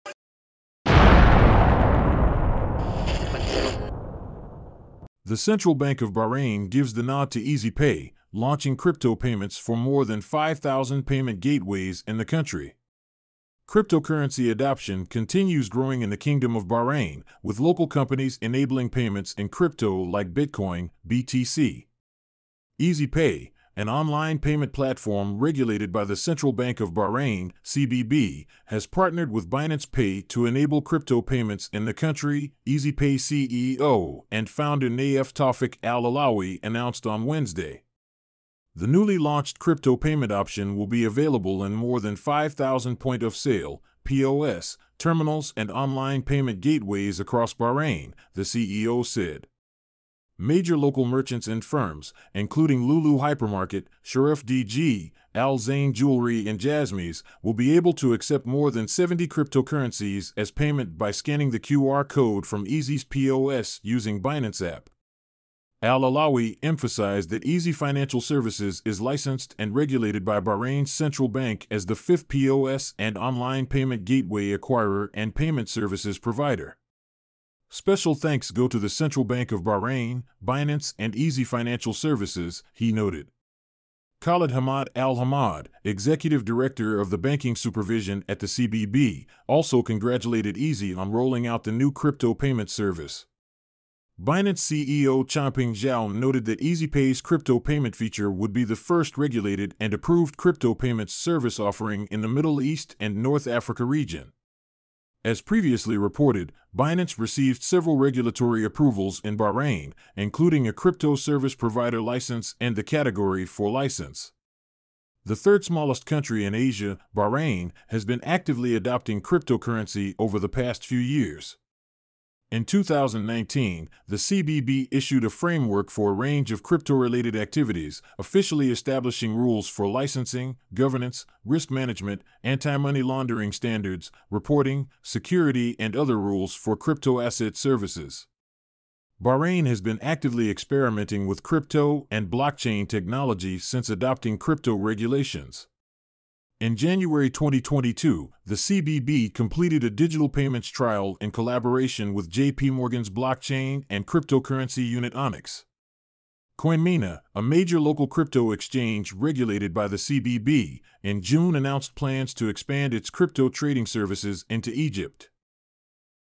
Regulated fintech in Bahrain enables crypto payments with Binance
EGLD: erd1uyzseaz3ne3eaf6r0j0p9nlrsyagfey4qnqv2j66pv3daggwj6sseu938d

DOT: 13MVMUApCciYR5HViE9wBHjc1wpmZpVbg1y8wDPuj6eN2JPD

BTC: 1KMoYwF9wU3FJ8h3TUWidTrAfav6ksCQyA

SOL: ACzxgHAhjzuwqee2anJQHzPx4iSm9GXrWNgbPTNpveQD